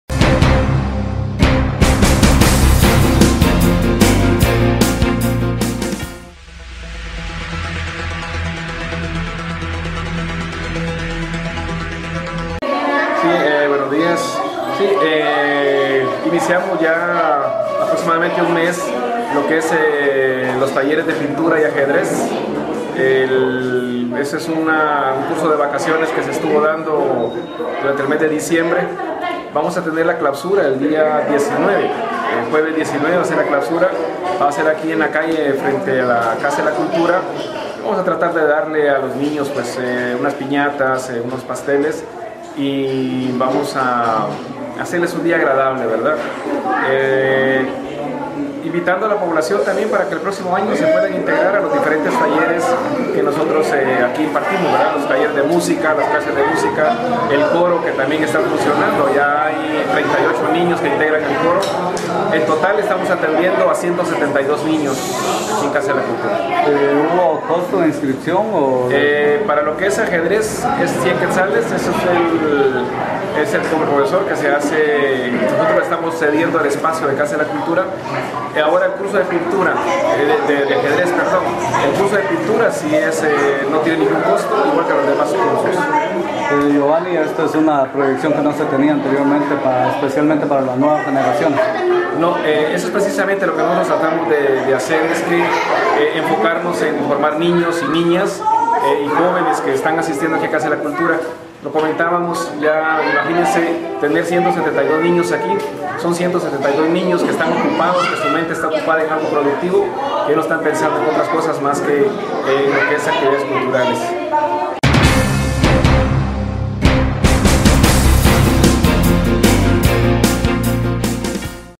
Taller Vacacional de Pintura para Niños
La Casa de la Cultura esta realizando un taller de pintura para niños para que aprovechen sus vacaciones, esto será durante el mes de diciembre.